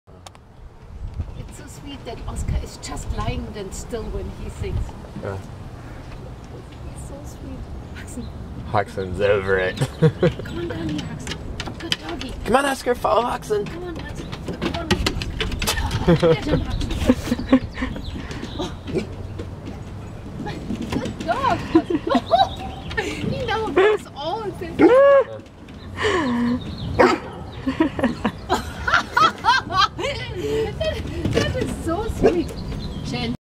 Oscar and Haxen Play (4/20/10)
glen of imaal terrier plays with a full grown lab/great dane mix.